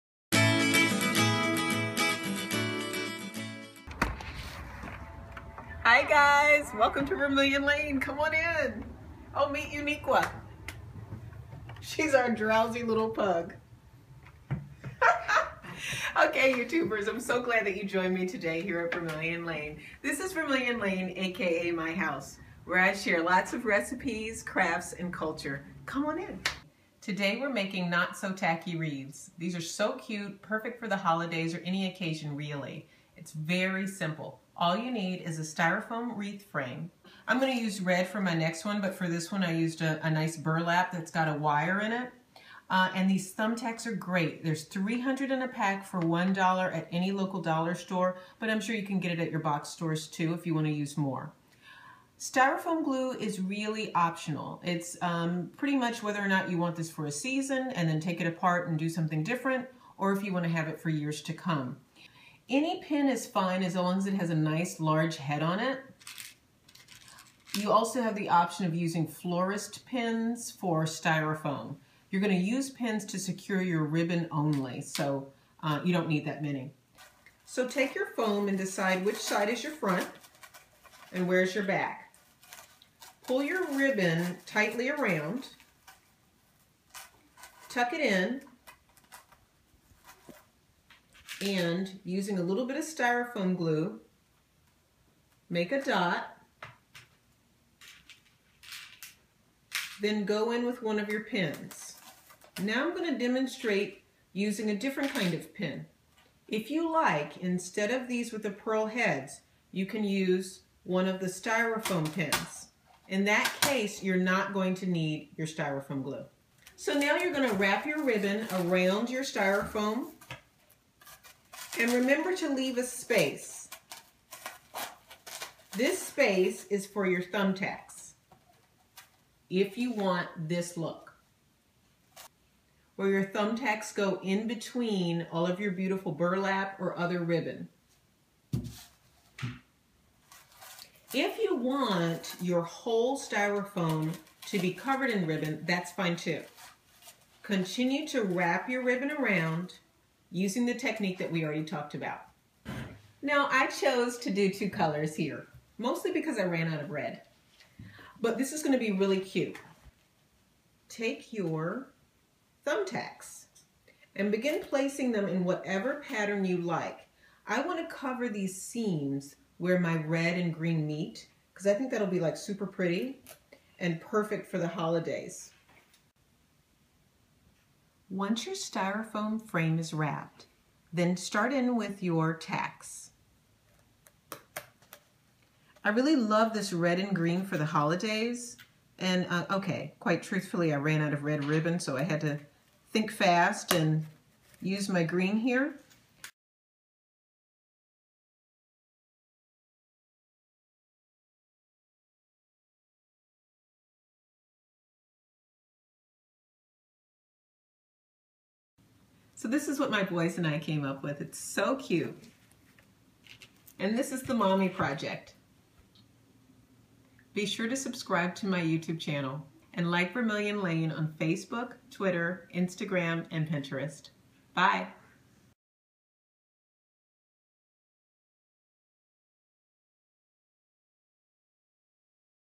How to make a Burlap & Thumbtack Wreath
Thumbtacks! Gorgeous gold holiday wreath. Super easy supplies: foam wreath frame, thumbtacks, burlap ribbon and pins.